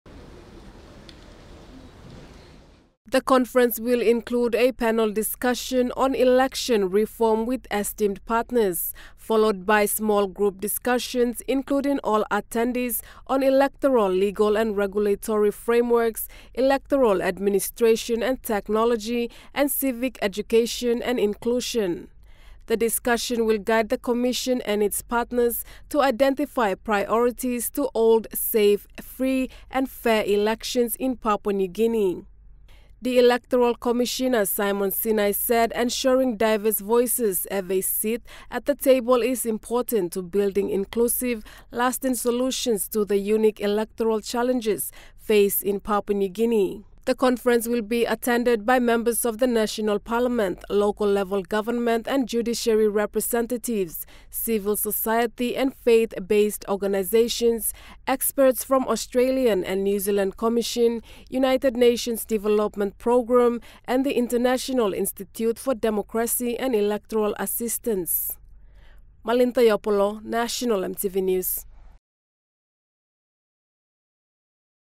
ELECTORAL REFORM CONFERENCE
Papua New Guinea Electoral commission will host the 2024 PNG Electoral Reform Conference on Tuesday, October 29th.

The conference will serve as a forum for stakeholders to assess the current state of electoral reforms in the country, gather insights from national and international experts and collaboratively develop strategies for implementing key reforms.